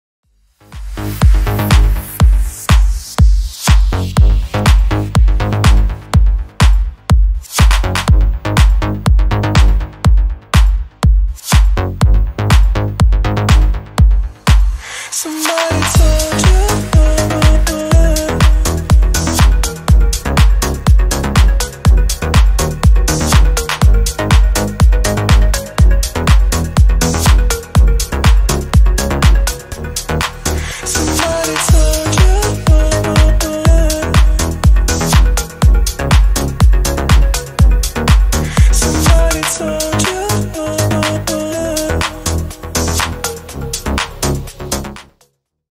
MMM, what a beautiful Slavic brunette!
"I am quite calm and modest girl) I believe that the best people are always self-confident and a little mysterious ... I am very selective about everything I do and with whom I do it .)"